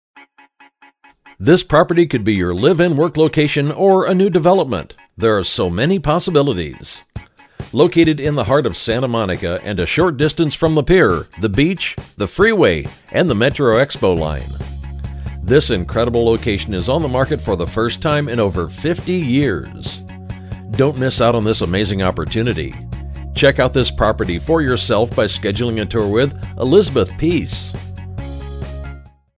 1124 Broadway Santa Monica, CA
1124 Broadway, Santa Monica, CA

This property could be your live and work location or a new development, there are so many possibilities. Located in the heart of Santa Monica and a short distance from the pier, beach, freeway and the Metro Yellow Line. This incredible location is on the market for the first time in over 50 years.